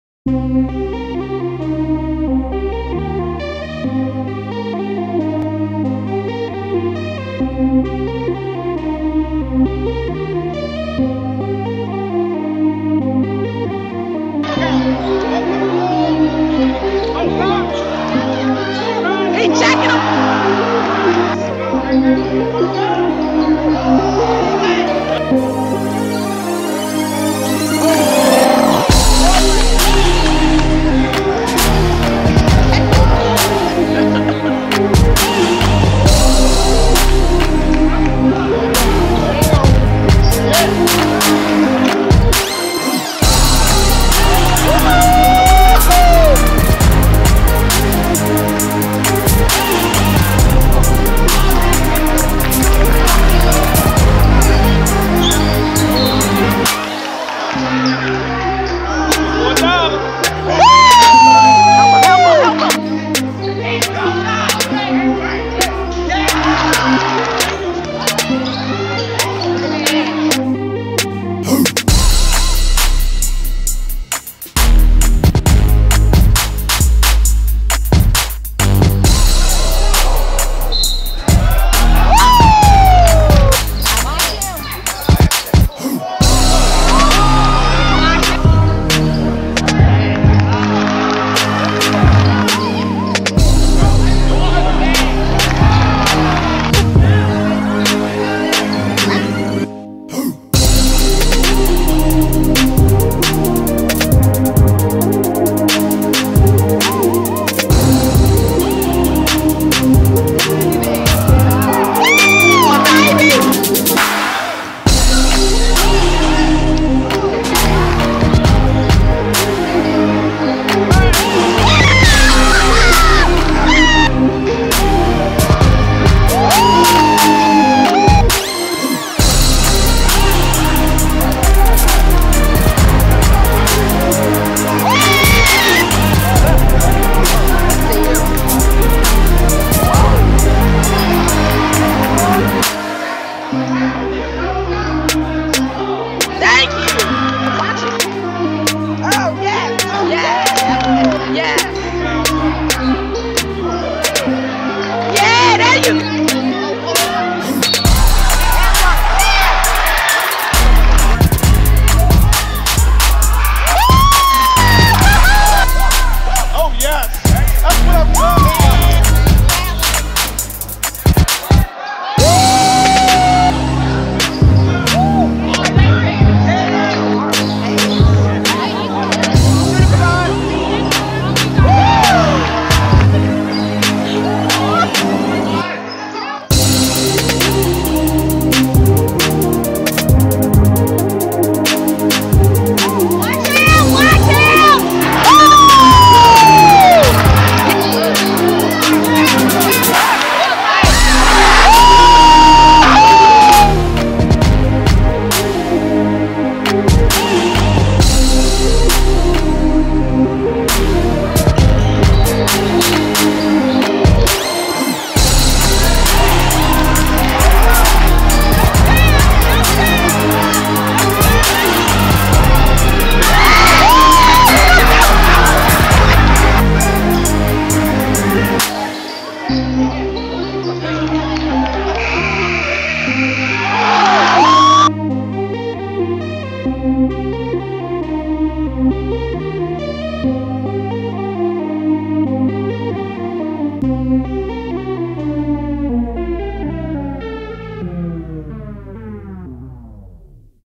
Koron Ford Senior Year highlights
Koron Ford - 2014 PG Sr. Owings Mills HS Senior Year Highlights